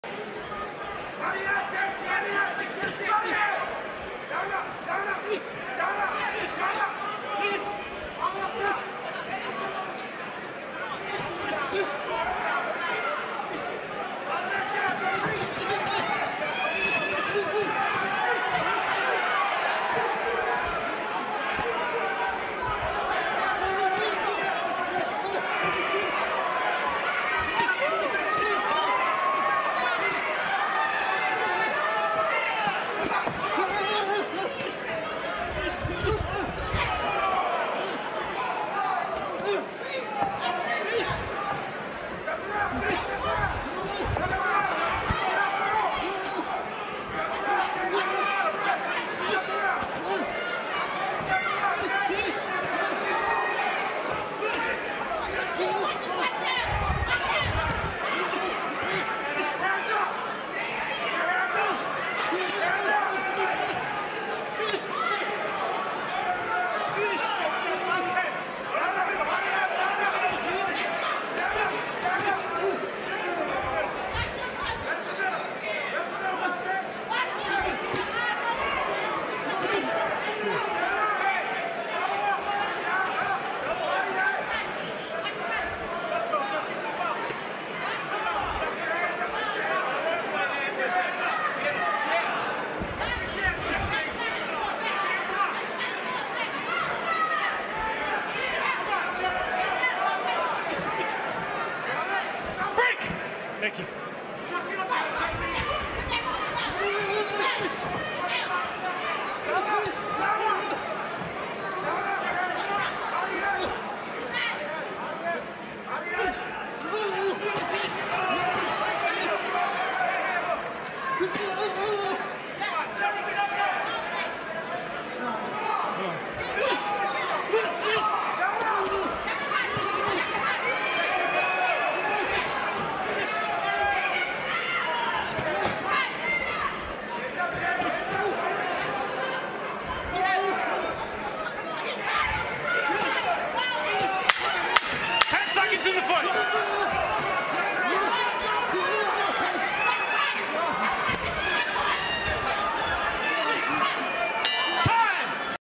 MY SECOND PROFESSIONAL BOXING MATCH!!!
Gerffred Ngayot (2-0) from buffalo steps in the ring with 18 professional fight veteran Harris at the Seneca Niagara Casino. Gerffred comes out victorious in a unanimous decision win over Harris.